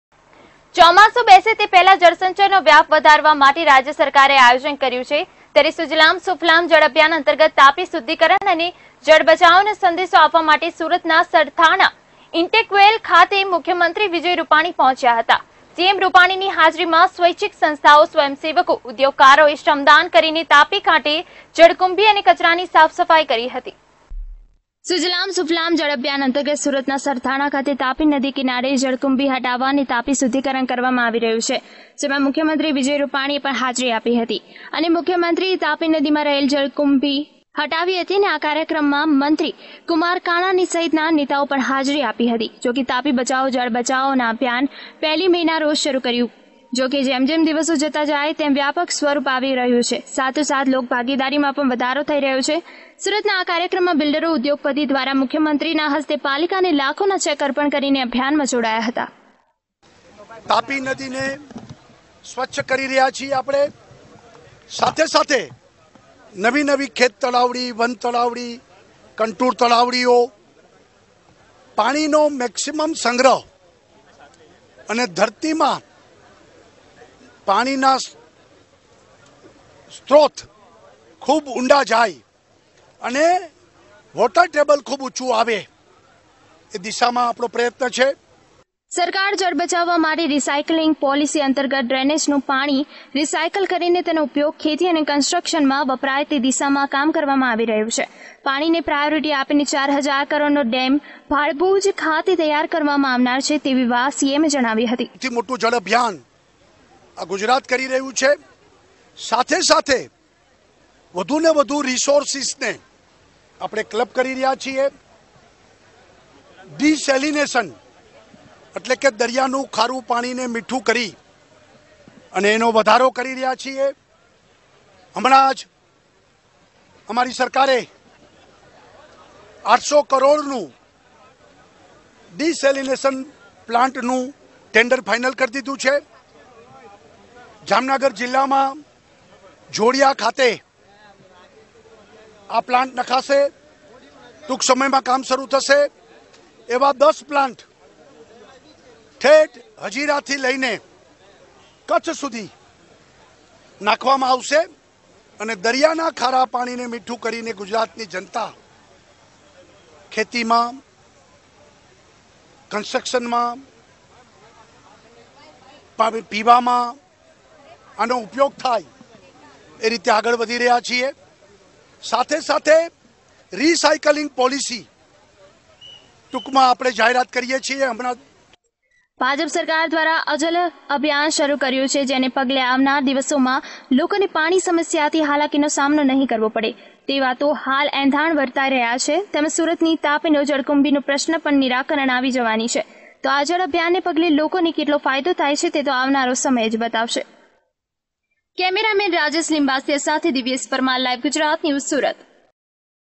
SURAT NA SARTHANA KHATE MANPA SANCHALIT YOJAYO KARYAKRAM
ચોમાસુ બેસે તે પહેલાં જળસંચયનો વ્યાપ વધારવા માટે રાજ્ય સરકારે આયોજન કર્યું છે..ત્યારે રવિવારના રોજ જે સુજલમ સુફલામ જળ અભિયાન અંતર્ગત તાપી શુદ્ધિકરણ અને જળ બચાવોનો સંદેશો આપવા માટે સુરતના સરથાણા ઇન્ટેકવેલ ખાતે મુખ્યમંત્રી વિજય રૂપાણી પહોંચ્યા હતા..સીએમ રૂપાણીની હાજરીમાં સ્વૈચ્છીક સંસ્થાઓ,સ્વયંસેવકો,ઉધોગકારોએ શ્રમદાન કરીને તાપી કાંઠે જળકુંભી અને કચરાની સાફસફાઈ કરી હતી...